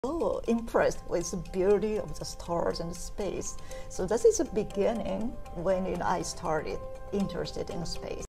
Japan's second female astronaut celebrates her mission
Naoko Yamazaki, the second Japanese female astronaut, captivated me with her narratives. Her fascination with the cosmos blossomed during her childhood in Hokkaido, where the star-studded night sky mesmerized her. She pursued aerospace engineering at Tokyo University and advanced to the master's course. For her first venture abroad, Naoko spent a year as an exchange student at the University of Maryland. While working at JAXA, she bravely applied for the astronaut program and successfully navigated the selection process alongside two male colleagues. The gripping account of her rigorous two-year training and exhilarating 15-day space journey is truly inspiring. Naoko's perception of space and Earth underwent a remarkable transformation, and she aspires to one day educate children at a school on the Lunar Station!